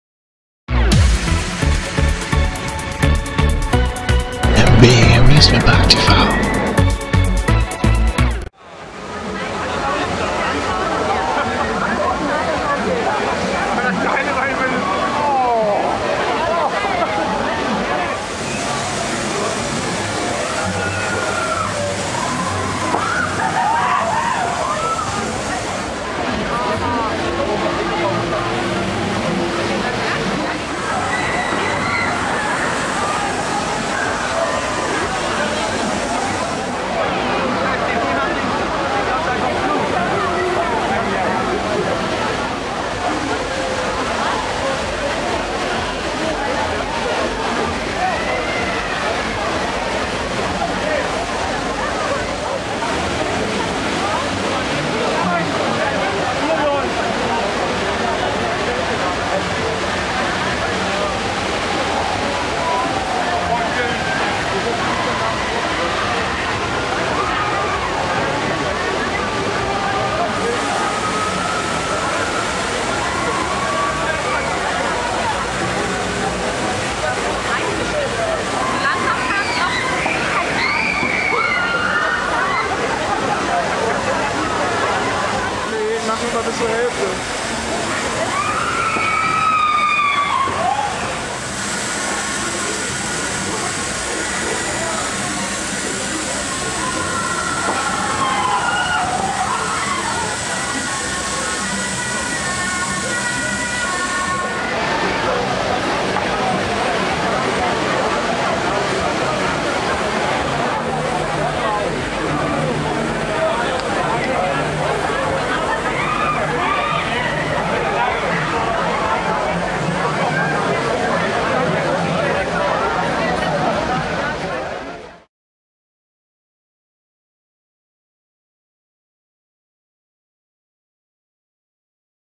Höllenblitz / Renoldi - Cranger Kirmes 2018
Moin! Der Höllenblitz vom Bremer Schausteller Klaus Renoldi jr. ist die größte Transportable Indoorachterbahn der Welt. Diese Anlage wurde 1992 gebaut und hatte in ihrer Laufbahn schon so einige "Outfits". Ursprünglich als Magic Mountain und ab der Kirmessaison 1998 als Star World. In der Winterpause 2006/07 erfolgte der letzte Umbau zum heutigen Höllenblitz. Gerstlauer Rides verlängerte hierzu die erste Abfahrt der Bahn bis ganz nach unten. Vorher führte die abfahrt lediglich bis zum Bahnhofsdach.
Höllenblitz besitzt Spinning Züge die auf 820m Fahrstrecke bis zu 80 Km/h erreichen. Höllenblitz ist als Safariblitz bis ende August 2019 im Serengeti Park Hodenhagen zu finden. Im Anschluss gehts zum Oktoberfest nach München. Viel Spaß mit den kleinen Offride Aufnahmen!